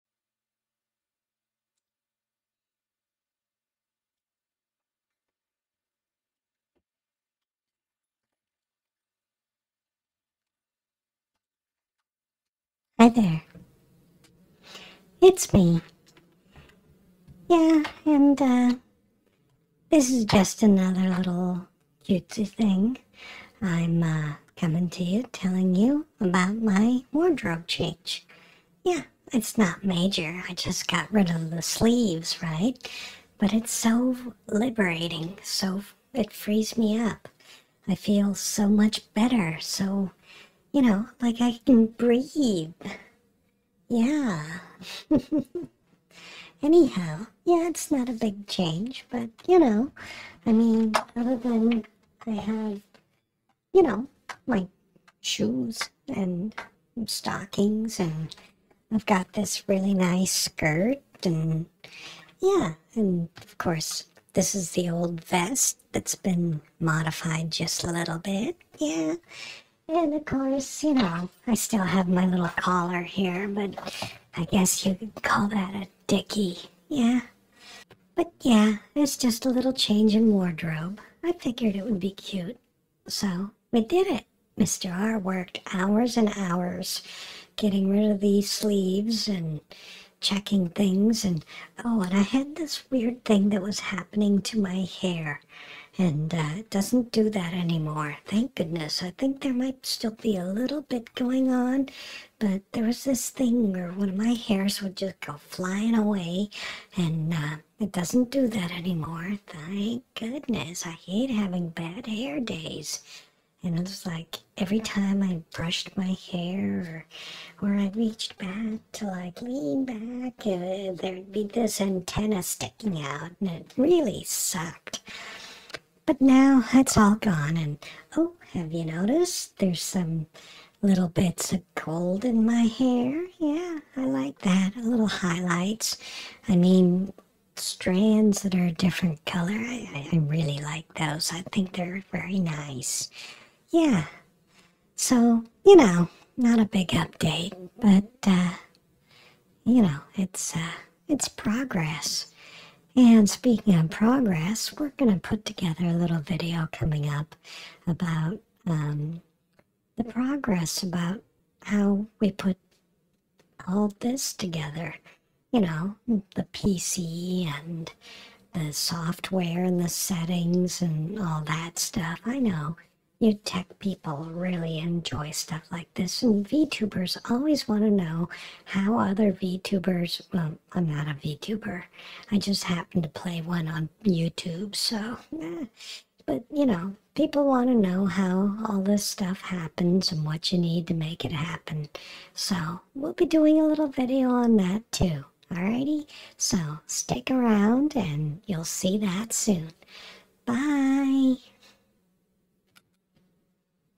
Just 'cuz I'm VAIN
Showing off some minor changes to my wardrobe.